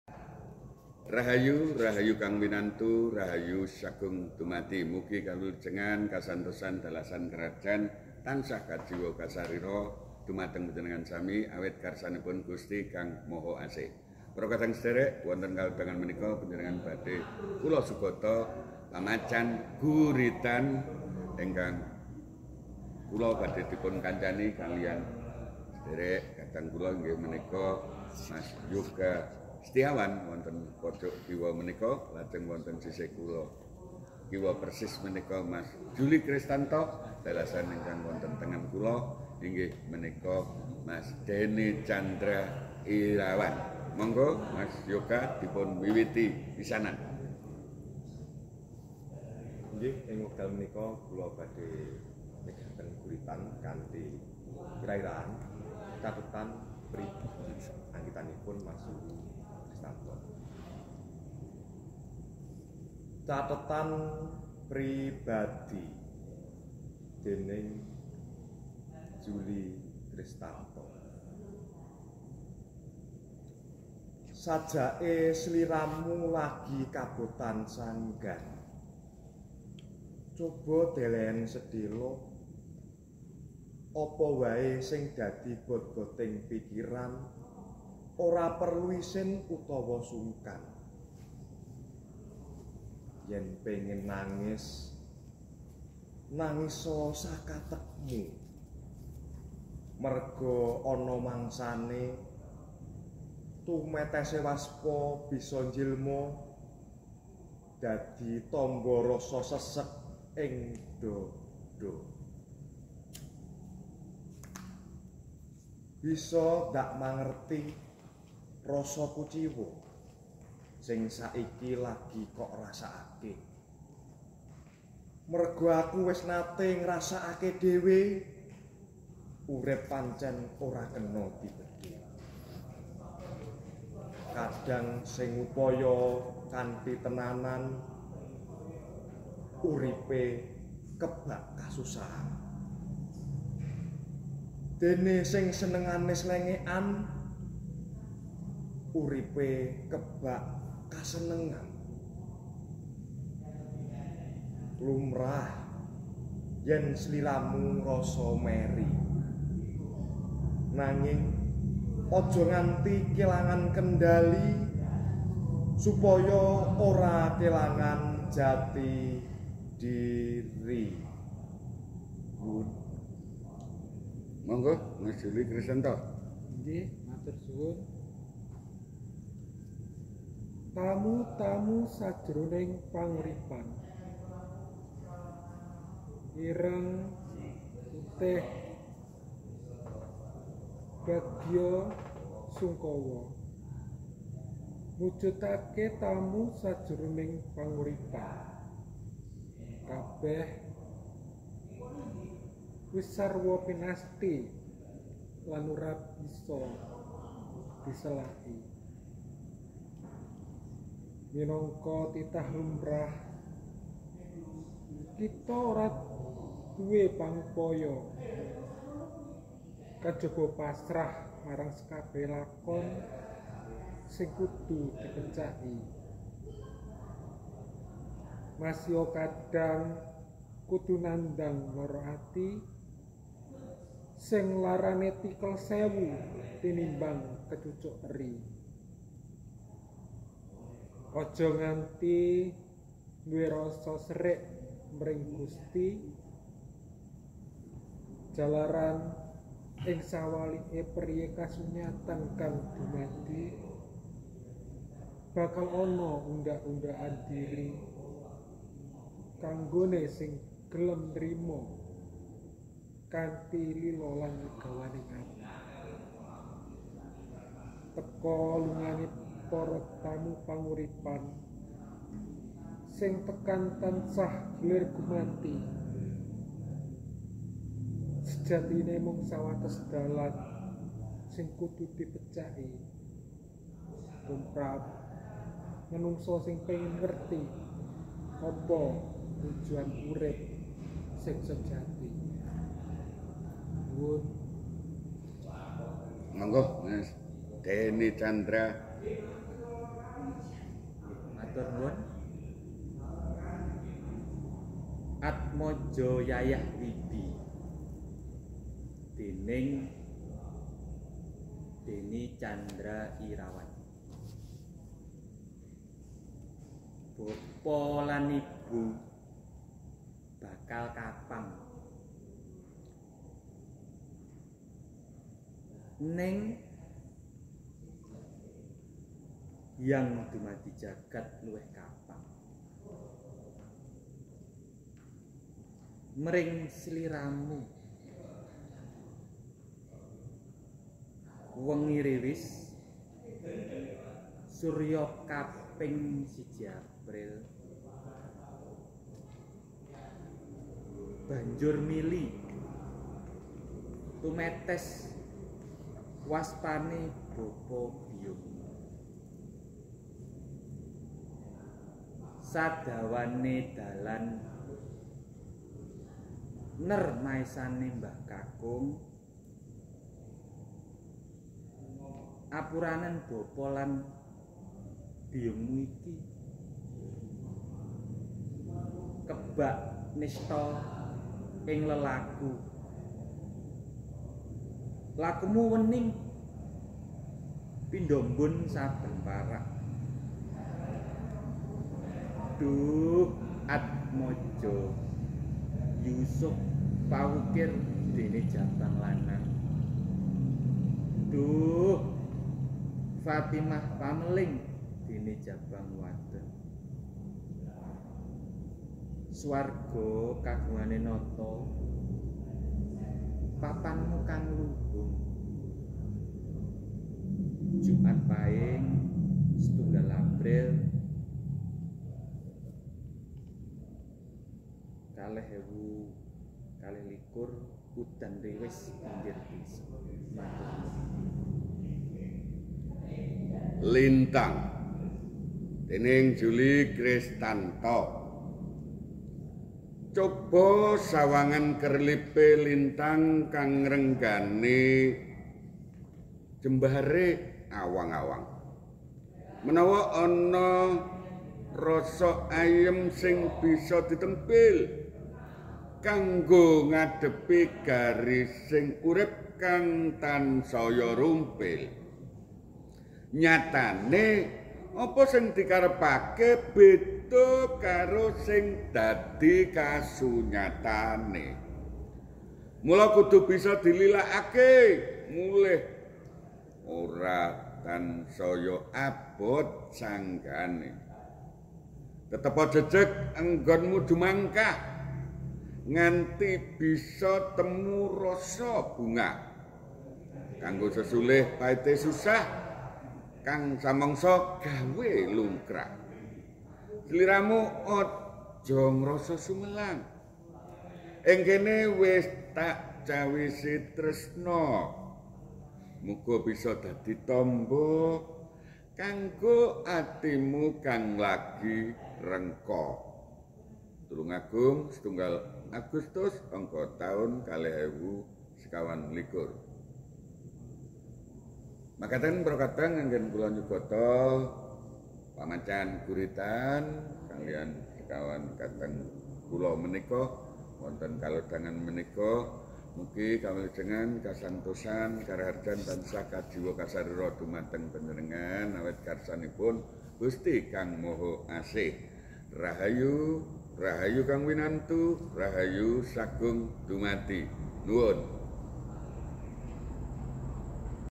MÅCÅ GURITÁN ÌNG WÊNGI ÁCÅRÅ TÉTRÅWÅRÅ SÁNGGÁR TRIWIDÅ
PÁMÁCÁN GURITÁN ÌNG WÊNGI ÁCÅRÅ TÉTRÅWÅRÅ SÁNGGÁR TRIWIDÅ
Pêndhåpå Ágêng Hánd Ástá Sìh Srêngát Blitár,
Málêm Ákád Lêgi 29 Sèptèmbêr 2024ÁM

CATHETHAN PRIBADI
Dening : Zuly Kristanto

Sajake sliramu lagi kabotan sanggan
Coba dhelehen sedhela
Apa wae sing dadi bot-boting pikiran
Ora perlu isin utawa sungkan
Yen pengin nangis
Nangisa sakategmu
Merga ana mangsane
Tumetese waspa bisa njilma
Dadi tamba rasa seseg ing dhadha

Bisa dakmangerti rasa kuciwa
Sing saiki lagi kokrasake
Merga aku wis nate ngrasakake dhewe
Urip pancen ora kena dibedhe
Kadhang sing ngupaya kanthi tenanan
Uripe kebak kasusahan
Dene sing senengane slengekan
Uripe kebak kasenengan

Lumrah yen sliramu ngrasa meri
Nanging aja nganti kelangan kendhali
Supaya ora kelangan jati dhiri
Tetepa sadhar sarta yakin lamun Gusti
Kagungan pesthi kang manungsa ora bisa weruhi

Tulungagung, 1 Agustus 2024

TAMU-TAMU SAJRONING PANGURIPAN
Dening : Zuly Kristanto

ireng-putih, bagya-sungkawa
mujudake tamu sajroning panguripan
kabeh wis sarwa pinasthi
lan ora bakal kena diselaki

minangka titah lumrah
kita ora duwe pangupaya kajaba pasrah
marang sekabehane lakon sing kudu dipecaki
masia kadhang kudu nandang lara ati
sing larane tikel sewu tinimbang kecucuk eri
aja nganti duwe rasa serik mring Gusti
jalaran ing sawalike perihe kasunyatan kang dumadi
bakal ana undhak-undhakan dhiri
kanggone sing gelem nrima kanthi
lila lan legawaning ati

teka lungane para tamune panguripan
sing tekan tansah gilir gumanti
sejatine mung sawates dalan
sing kudu dipecaki
tumrap manungsa sing pengin ngerti
apa tujuan uripe kang sejati

ATMAJA YAYAH WIBI

Bapa lan Ibu Bakal Kapang
Ning Kang Hyang Dumadi isih Luwih Kapang Mring Sliramu
Wengi riwis surya kaping siji April
Banjur mili tumetes waspane Bapa Biyungmu
Sadawane dalan ner maesane Mbah Kakung
Apurana Bapa lan Ibumu iki
Kebak Nistha ing lelaku
Lakumu Wening kadya Tumetese Mbun saben Parak
Dhuh Atmaja Yusuf Pawukir Dene Jabang Lanang, Fatimah Pameling Dene Jabang Wadon
Swarga Kagungane Nata Papanmu Kang Luhung

Jumat Pahing, 29 Say'ban, Candra Kaping 1 April 2022, Tabuh 9 Aridalu, Wuku Shinta, Ngepasi Udin Riwis Pinggir Desa Graha Husada Trisna Medika

LINTANG
Dening : Zuly Kristanto

Coba sawangen kerliping lintang
Kang ngrenggani jembare awang-awang
Menawa isih ana rasa ayem sing bisa ditempil
Kanggo ngadhepi garising urip kang tansaya rumpil

Nyatane apa sing dikarepake
Beda karo sing dadi kasunyatane
Mula kudu bisa dililakake
Murih ora tansaya abot sanggane

Tetepa jejeg anggenmu jumangkah
Nganti bisa kok temu rasa bungah
Kanggo sesulih paite susah
Kang samangsa gawe lungkrah

Sliramu aja ngrasa sumelang
Ing kene wis dakcawisi tresna
Muga bisa dadi tamba
Kanggo atimu kang lagi rengka

Blitár Sêtu Pòn, 29 Sèptèmbêr 2024
Pádhépòkán Wòng Ágùng (Wárgå orá ngáráh árêp gugát máráng Gùsti)
Singå Kêri (Siji Sångå Kêbònsári) Suråbåyå

Nuwùn.
“Sálám sêgêr wárás lán bungáh sumringáh!”
“Gùsti Káng Måhåwikán tánsáh párìng kánugráhán.”